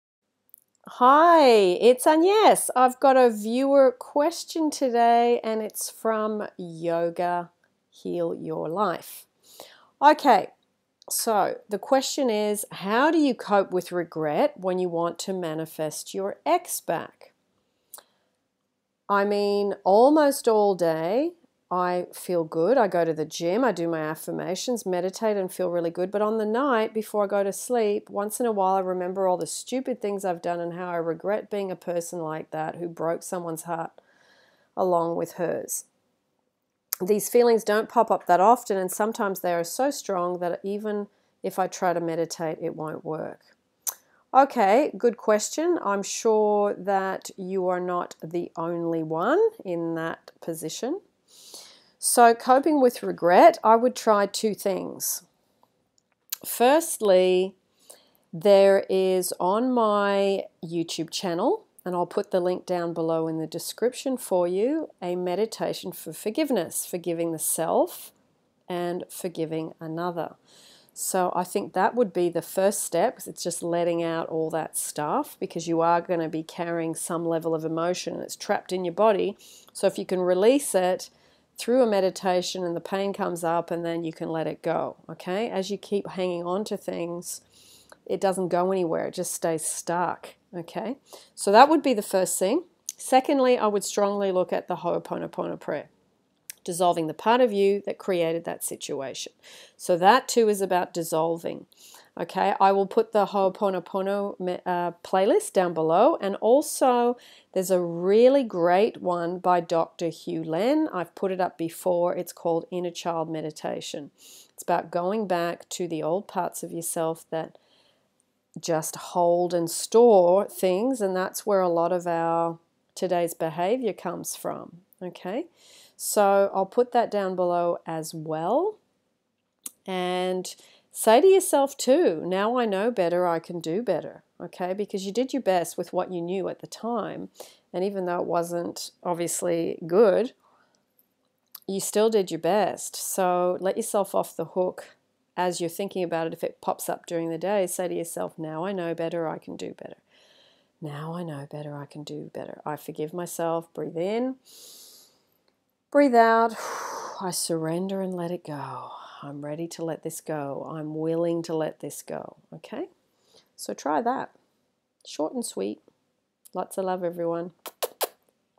How to Let Go of Regret
Viewer's Question About How to Let Go of Regret when you want to manifest your ex back?